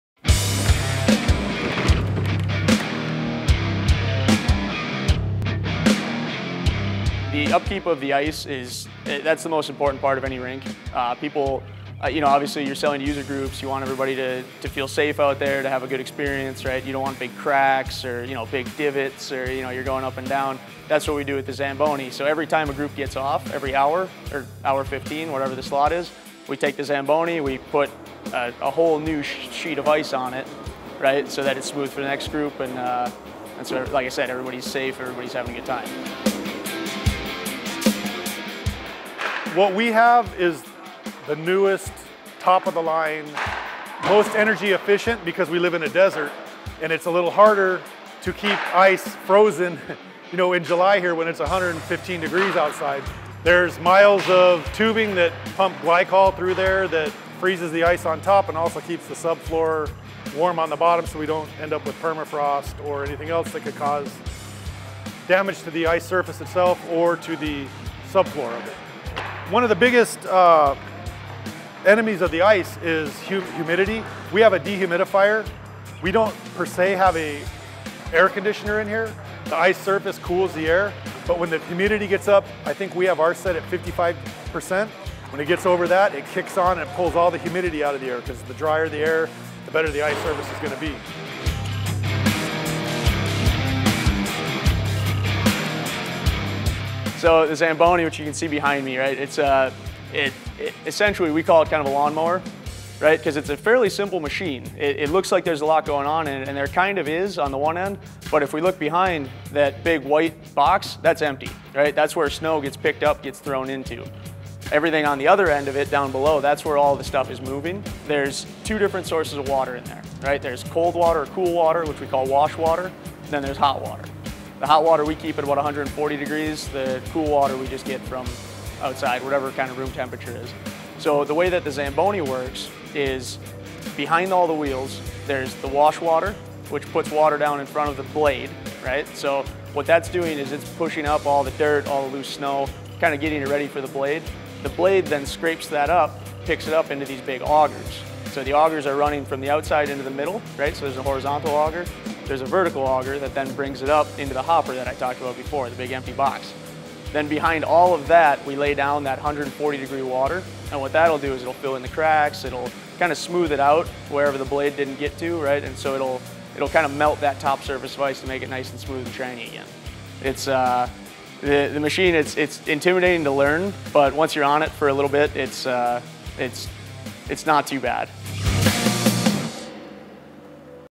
How an Ice Rink works
Su Vida visits and ice rink to learn what it takes to make ice in the desert. SVE187